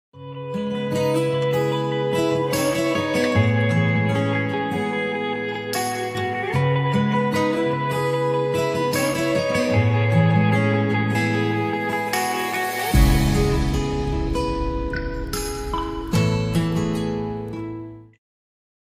Enjoy the feast of lights!
Enjoy the feast of lights! A wonderful drone show appeared in the night sky of Wuhan, central China's Hubei Province, displaying a masterpiece of modern technological artistry.